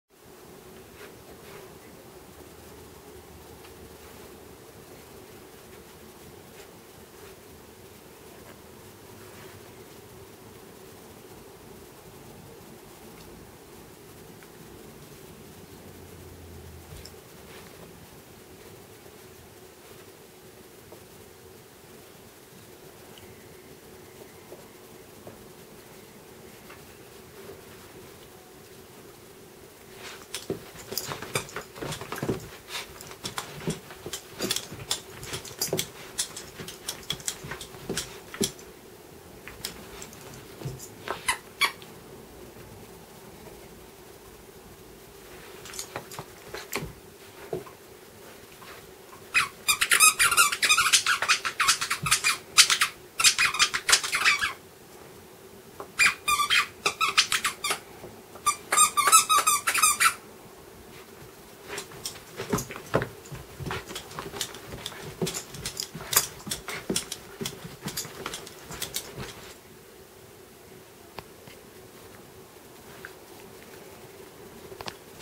Corgi Viciously Shakes His Squeaky Toy
Primo the Corgi gives his squeaky toy some tough love.